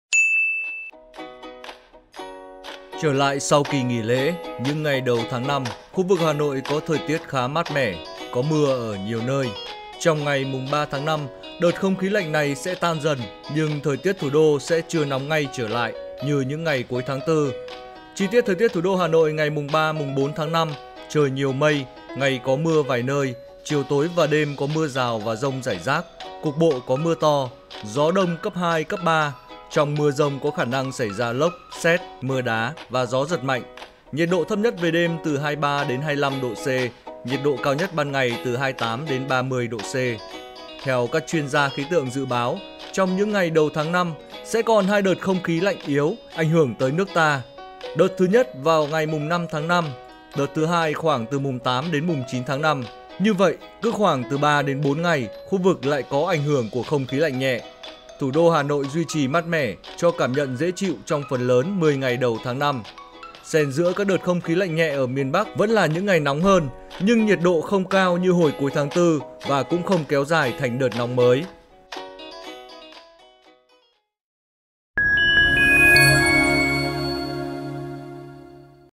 Những ngày đầu tháng 5, Hà Nội đón thêm hai đợt không khí lạnh | Tiền Phong TV
Tiền Phong TV | Theo dự báo từ Trung tâm Dự báo Khí tượng Thủy văn Quốc gia, trong thời gian đầu tháng 5 này thời tiết miền Bắc, khu vực Hà Nội khá mát mẻ do ảnh hưởng của không khí lạnh. Trong những ngày tới dự kiến khu vực sẽ đón thêm hai đợt không khí lạnh nữa.